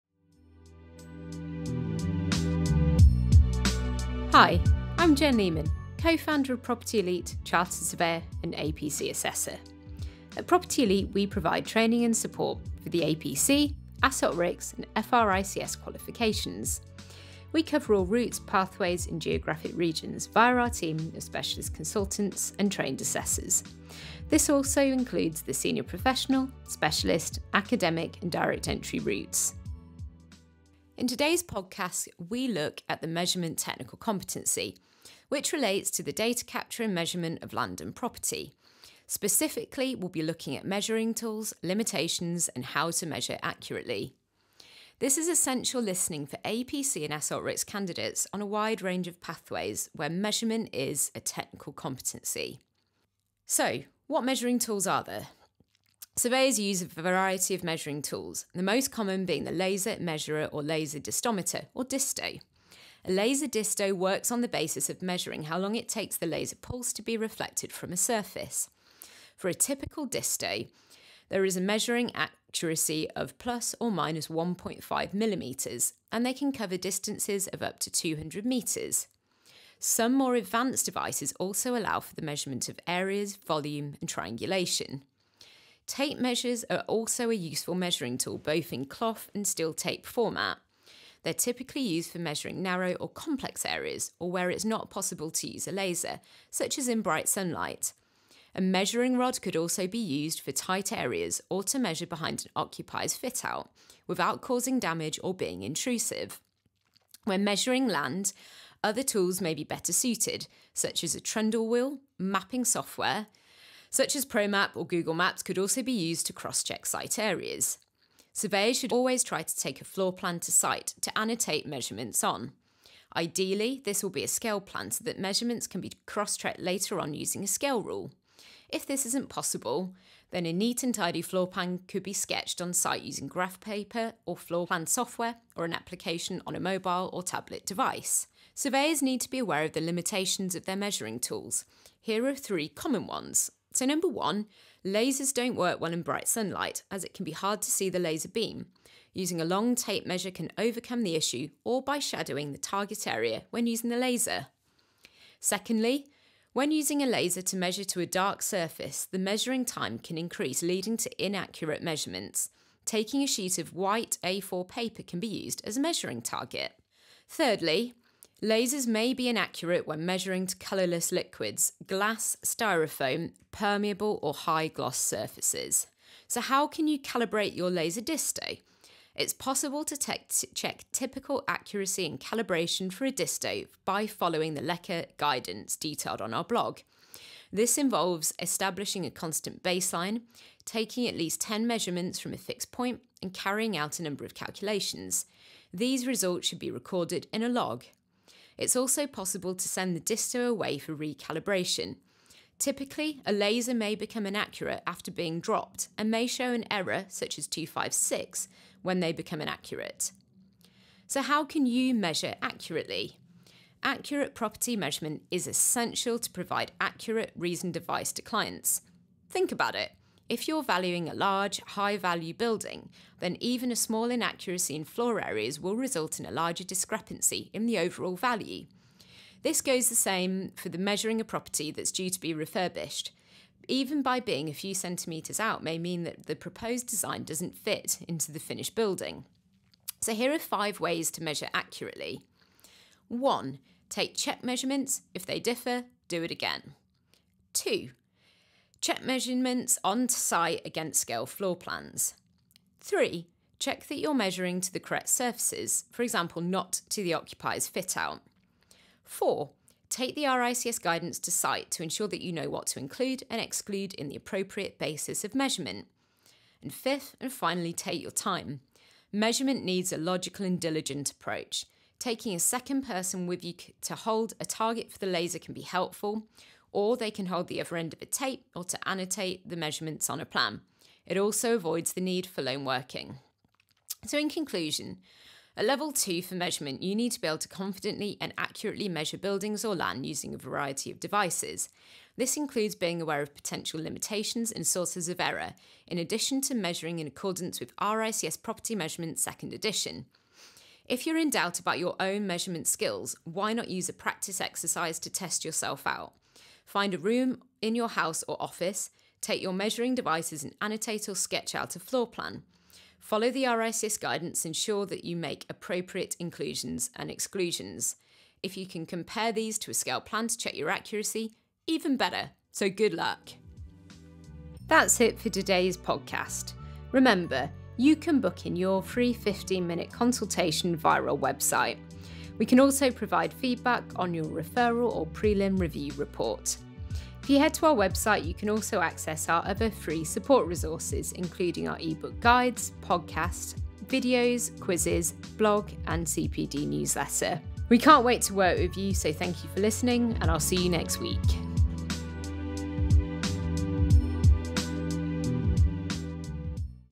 Hot Topic Highlight - Measurement Tools
This podcast discusses the Measurement technical competency, which relates to the data capture and measurement of land and property. Specifically, we will be looking at measuring tools, limitations and how to measure accurately.

It is essential listening for RICS APC and AssocRICS candidates on a wide range of pathways, including Environmental Surveying, Geomatics, Planning & Development, Land & Resources, Commercial Real Estate, Corporate Real Estate, Facilities Management, Residential, Valuation, Building Control and Building Surveying.